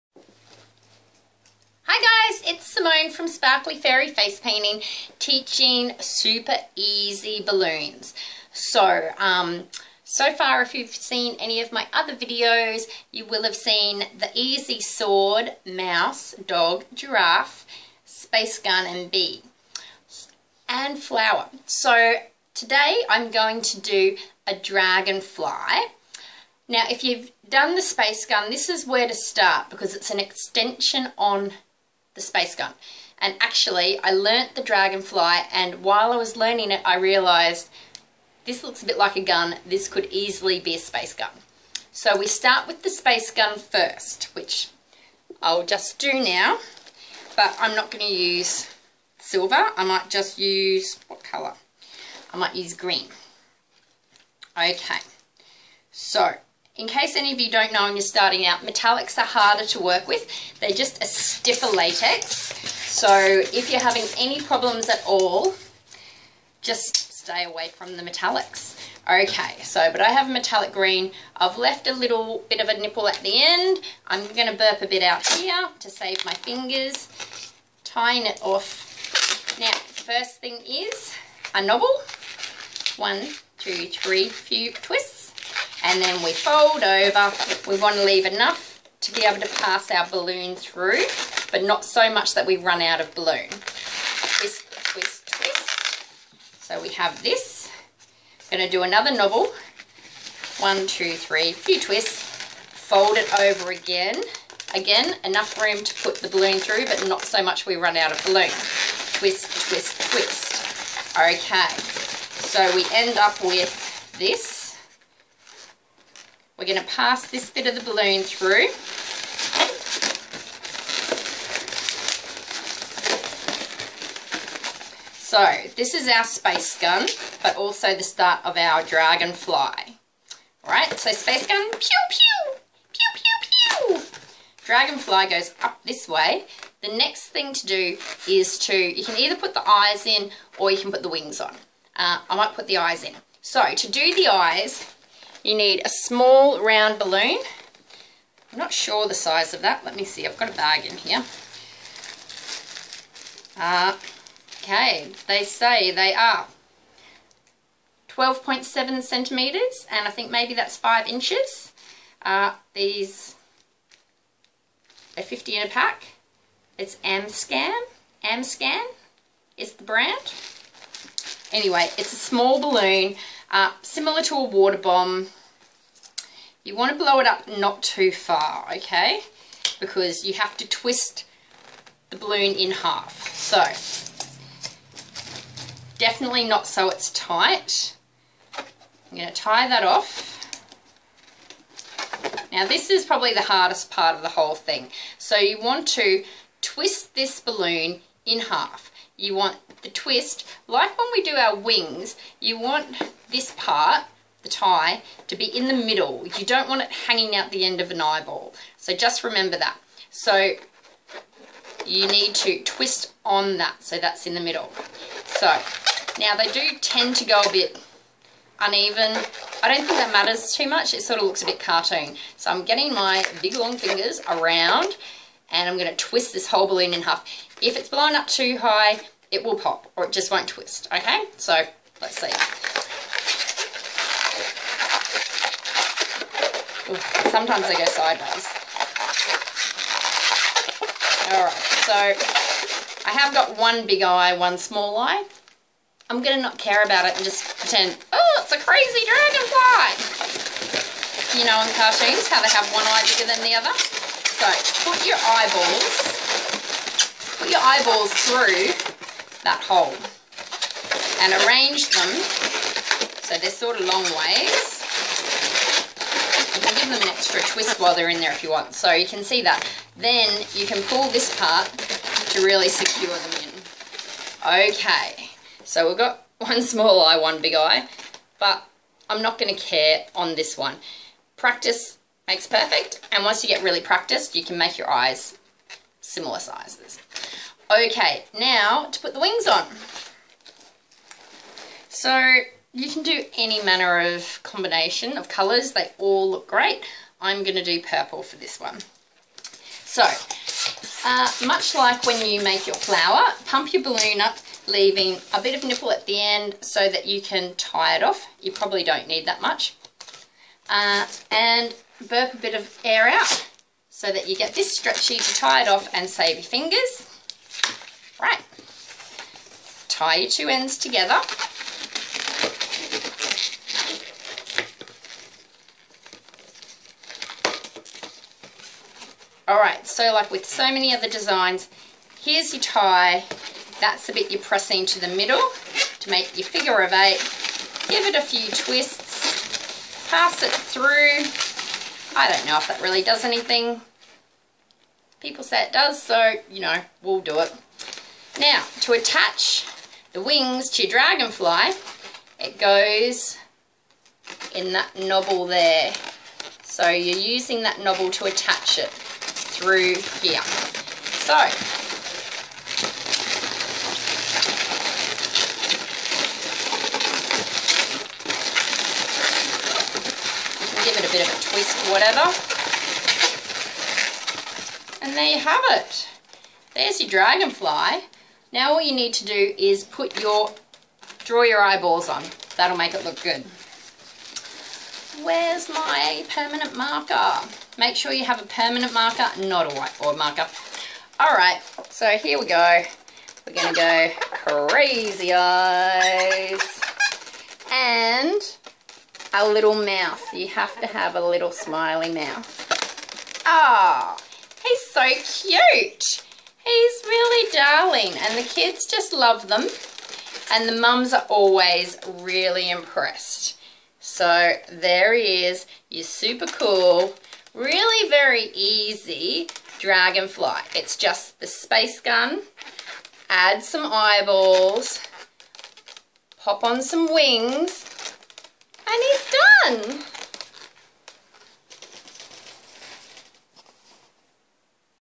Easy three balloon dragonfly.
This balloon creation is easier than it looks.

The first step is to make the space gun I have demonstrated in previous tutorial.

The second step is to add eyes. This takes a little practice.

The third step is to add wings.

The final step is to draw the face on with a permanent marker.

You will need 2 x 260 Q balloons and a small round balloon (like a water bomb).